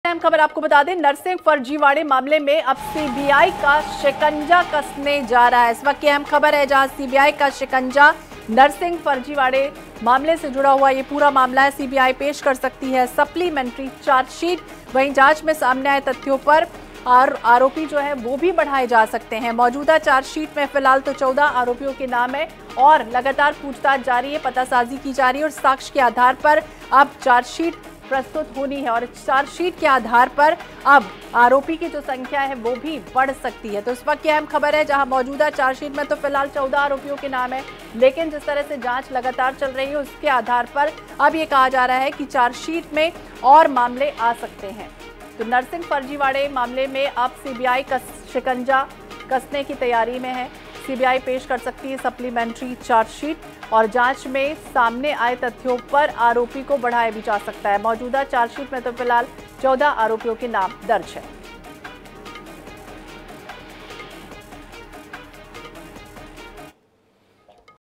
Nursing Scam मामले में CBI का शिकंजा, सीबीआई पेश कर सकती है सप्लीमेंट्री चार्जशीट
Incoming Search Terms: